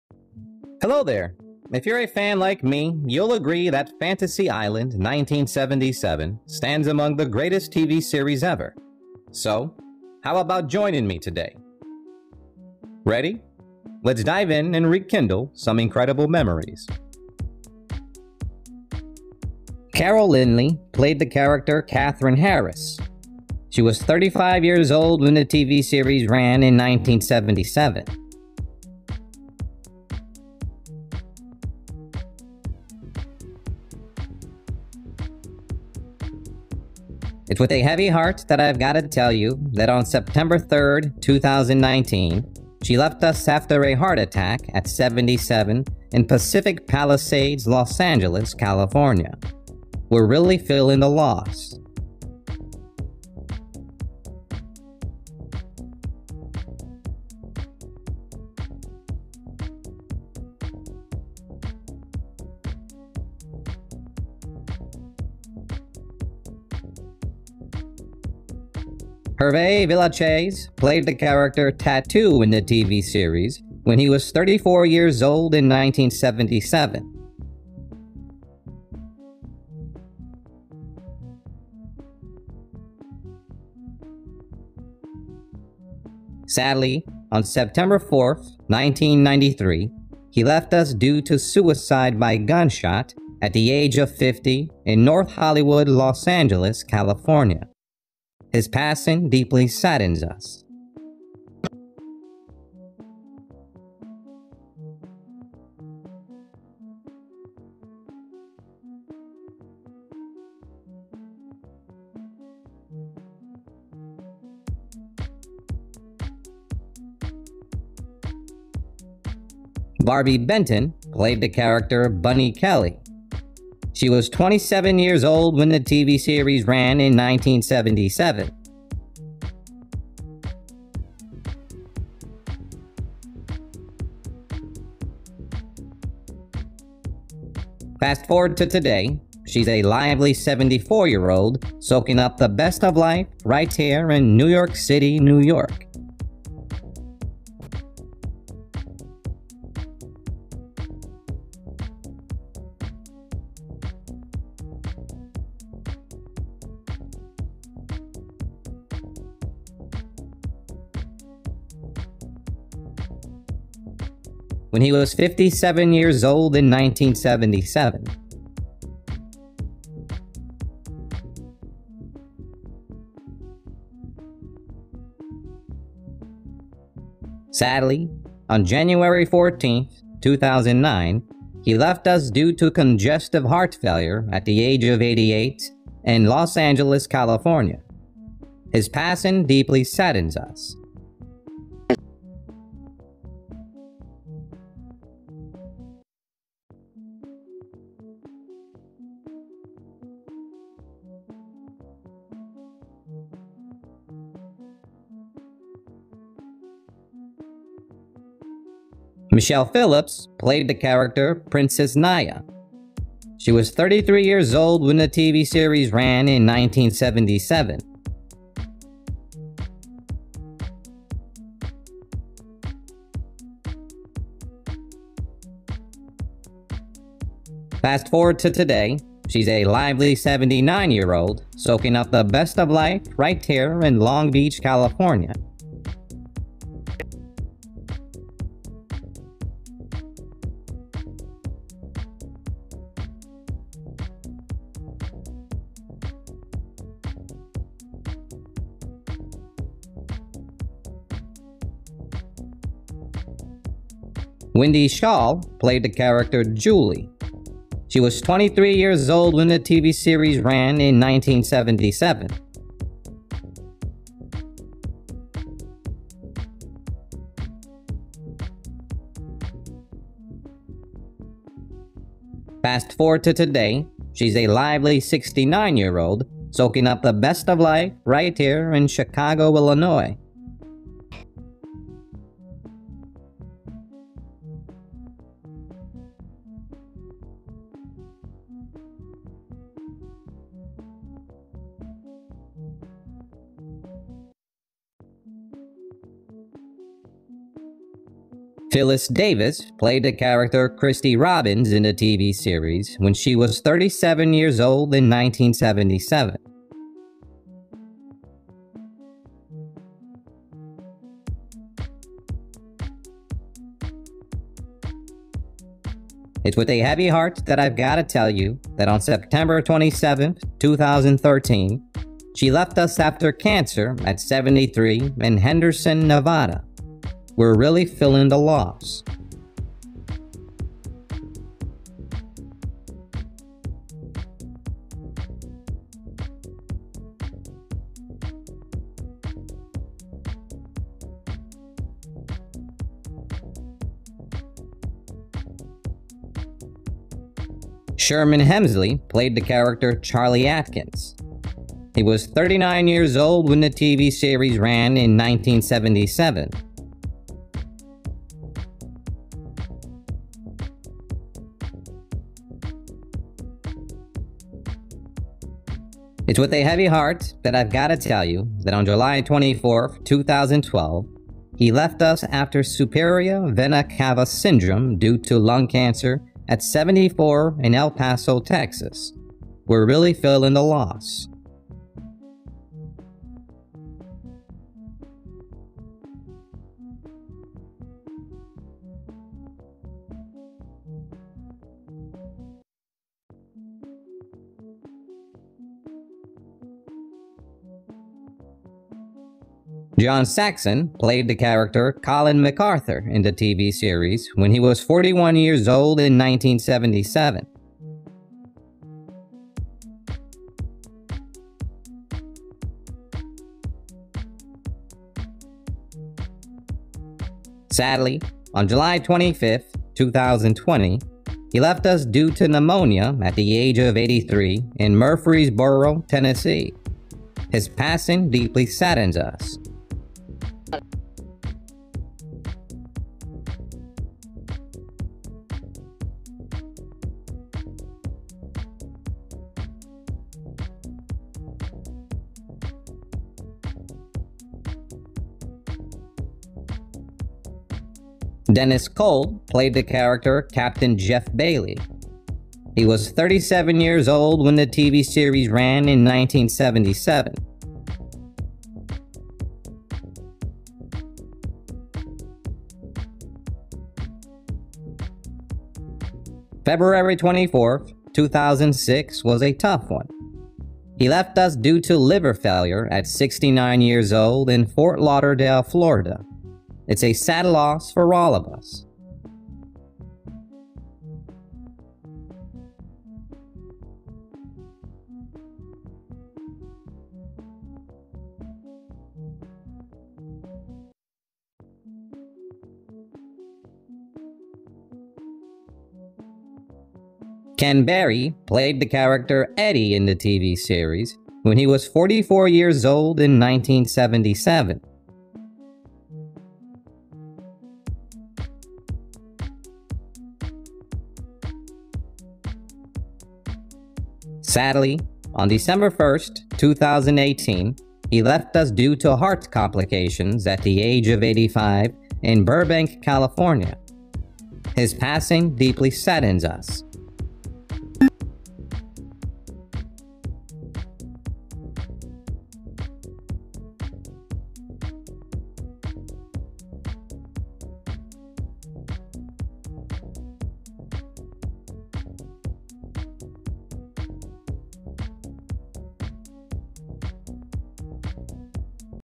FANTASY ISLAND (1977) Cast_ Then and Now 2024
FANTASY ISLAND (1977–1984) Cast_ Then and Now 2024
Fantasy Island TV Series (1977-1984) Then and Now 2019 Fantasy Island Stars "The Hosts" and some of the "Guests" through the years. All World Review Then and